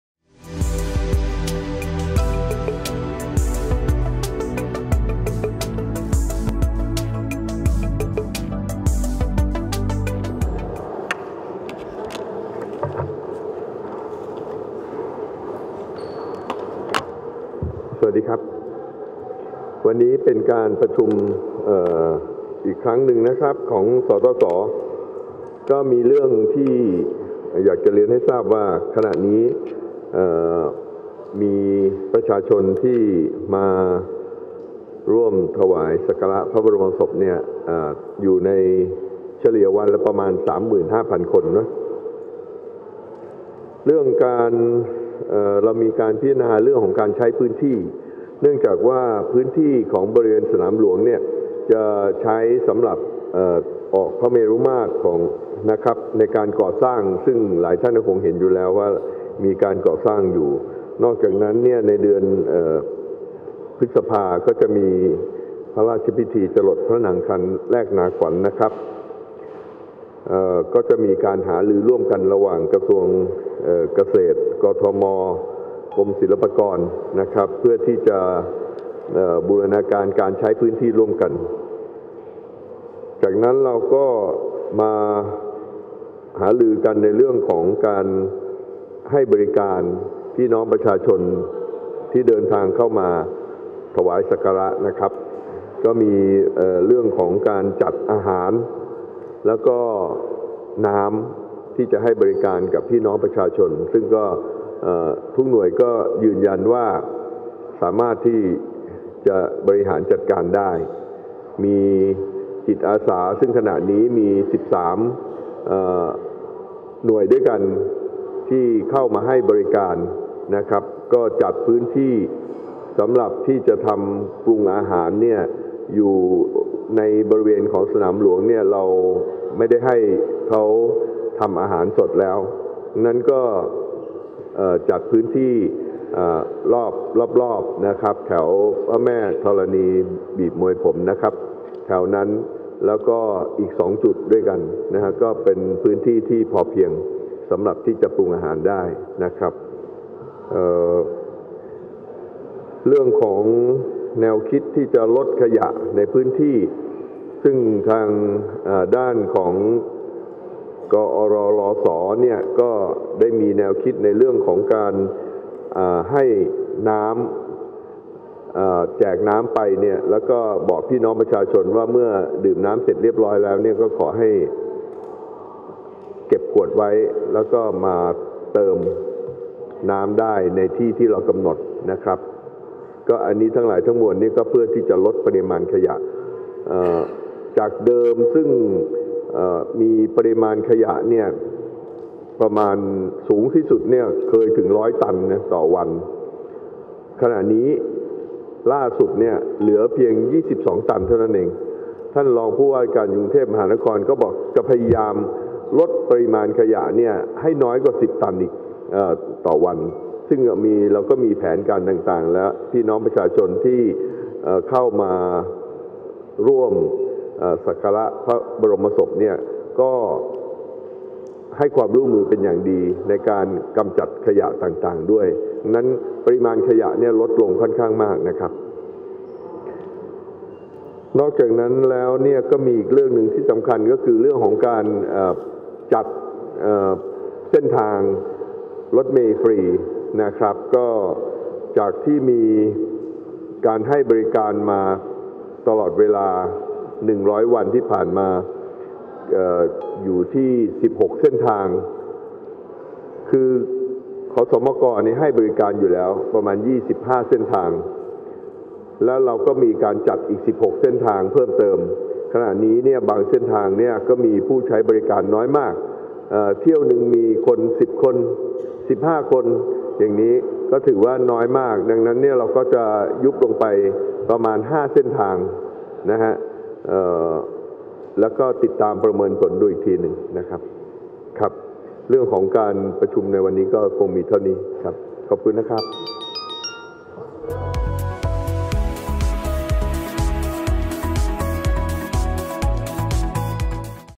รัฐมนตรีประจำสำนักนายกรัฐมนตรีแถลงการประชุมศตส
รัฐมนตรีประจำสำนักนายกรัฐมนตรี (นายออมสิน ชีวะพฤกษ์)
แถลงหลังการประชุมคณะกรรมการศูนย์บัญชาการติดตามสถานการณ์
ณ ตึกสันติไมตรี (หลังใน) ทำเนียบรัฐบาล